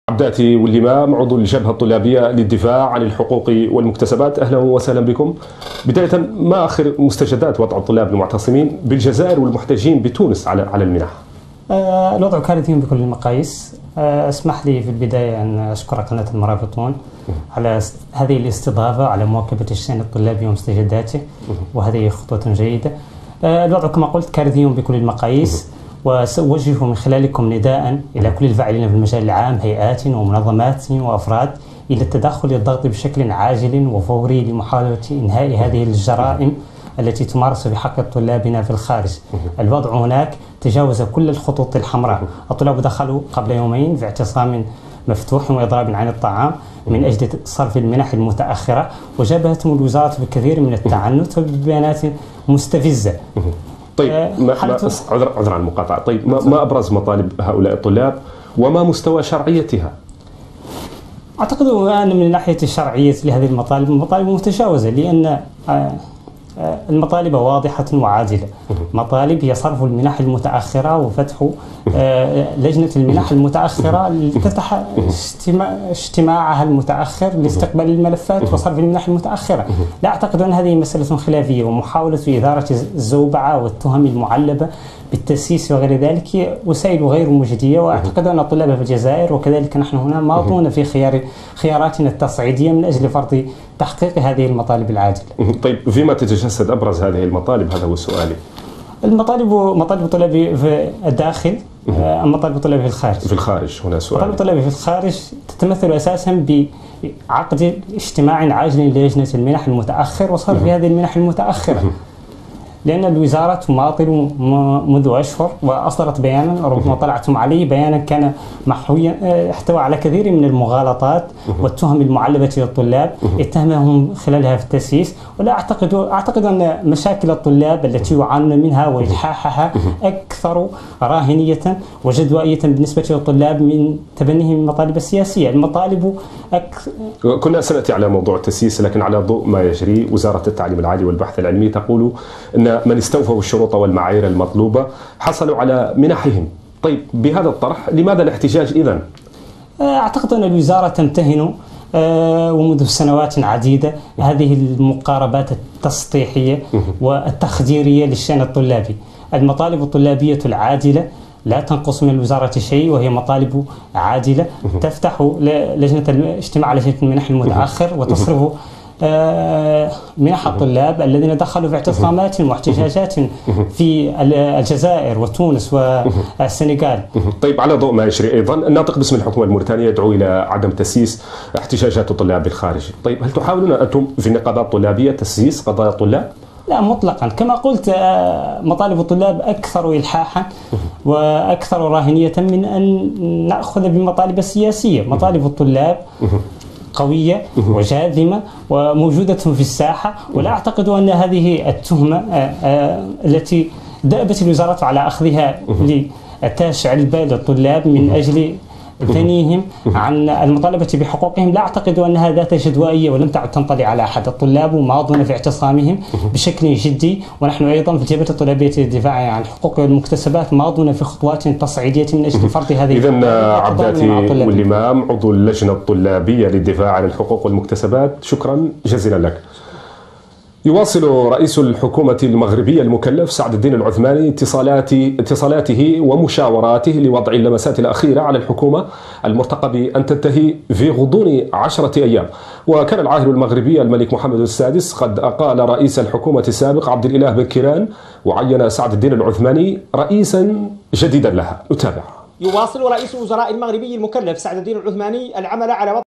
عضو الجبهة الطلابية عبداتي لمام ضيف نشرة المرابطون حول قضية الطلاب المحرومين من المنح
الجبهة الطلابية للدفاع عن الحقوق والمكتسبات حذرت من استمرار الوضع القائم المتمثل في حرمان الطلاب في الخارج من منحهم، وما قد يؤول إليه الوضع خصوصا في ظل لجوء بعضهم للإضراب عن الطعام
قناة المرابطون | تاريخ البث: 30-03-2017